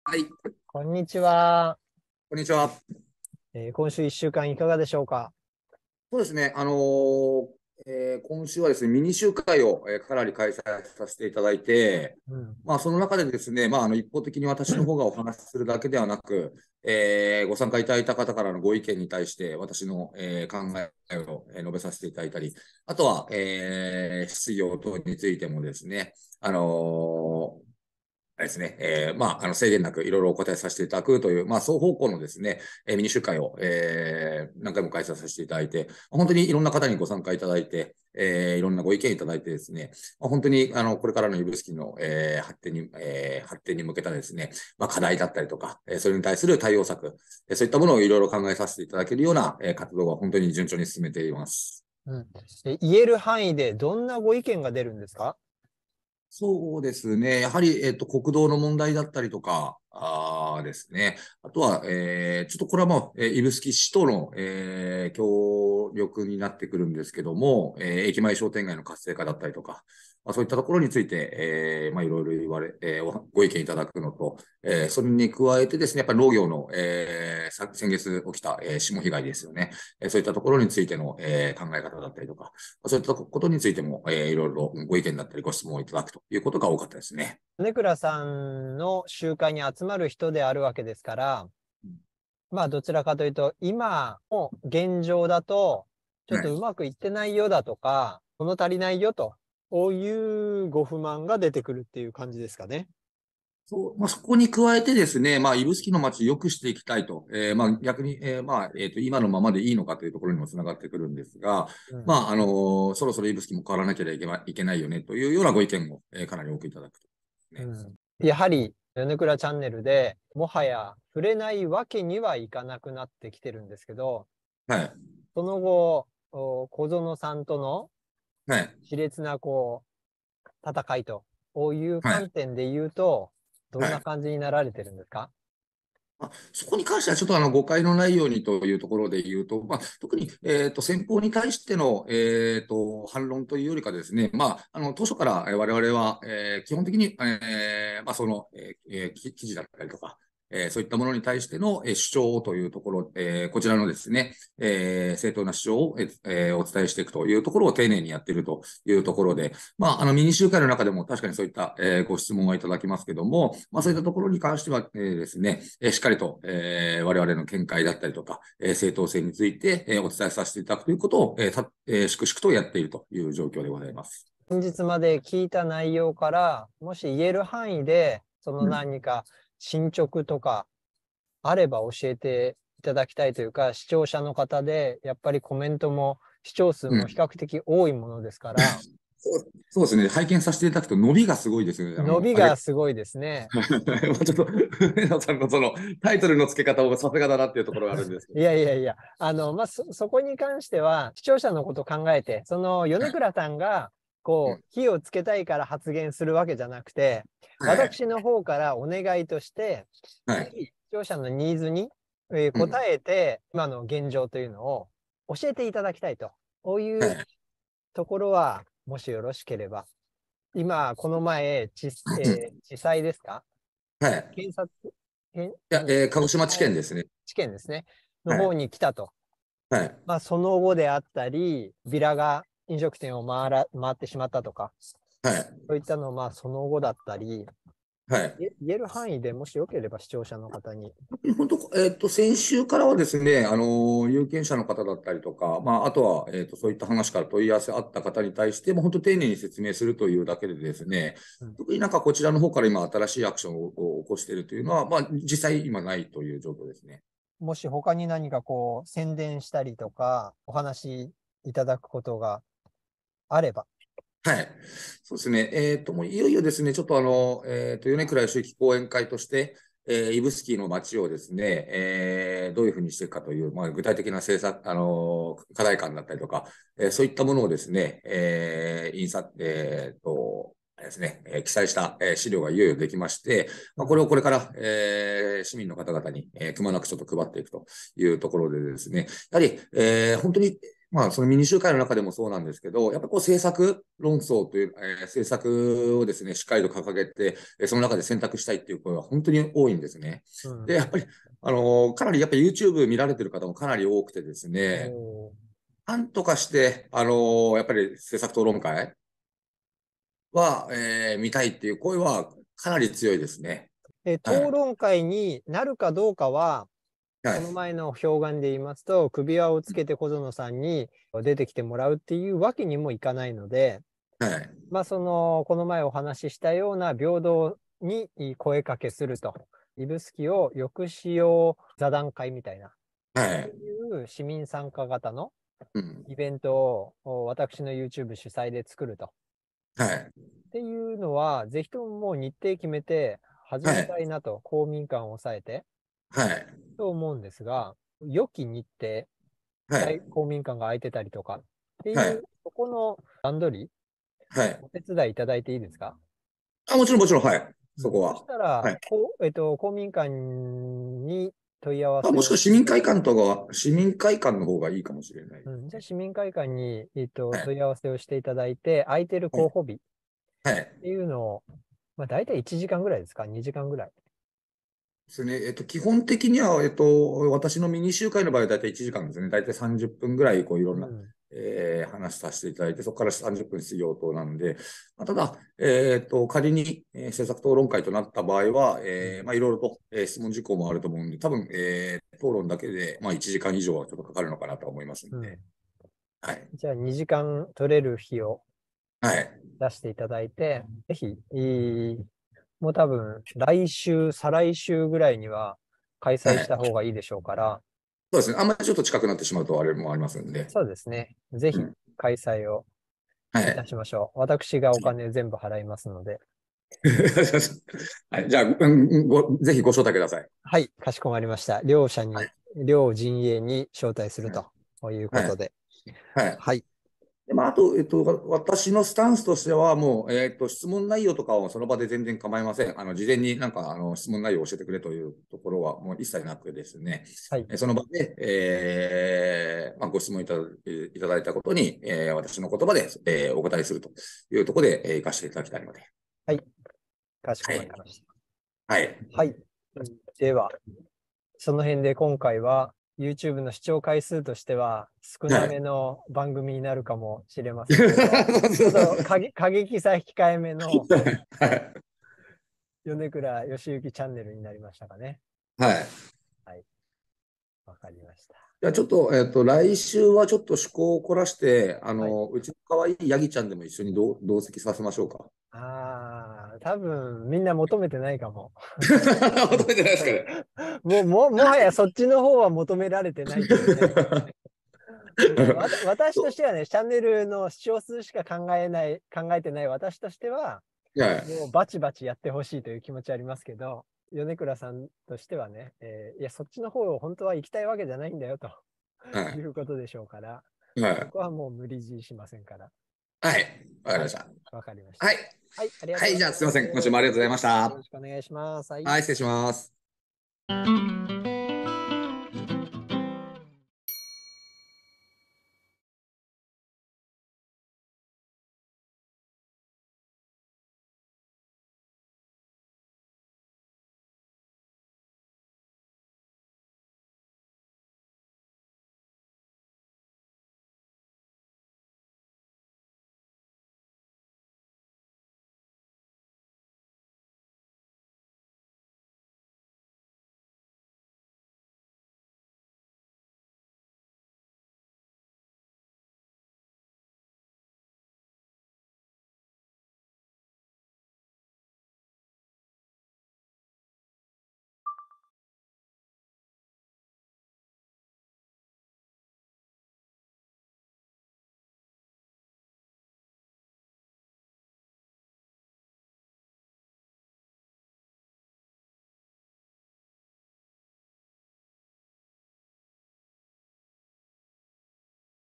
ノーサイドで語ろう「指宿を良くする座談会」学カフェが主催します🙋🏻‍♂️（2023年3月7日）
昨年の指宿市長選にも立候補した米倉よしゆきさん
今年4月の県議会議員選挙に立候補予定の米倉さんに毎週15分お話を伺う定例収録

当番組は、公平中立な立場から取材を進めます。ただいま、小園県議にもインタビューの申し入れをしておりますので、今回の番組は、米倉よしゆきチャンネルとしてのご意見となります。
視聴者からのコメントを元に今後両者インタビューを進めて参りたいと思っておりますので、どうぞどしどしコメントくださいませ。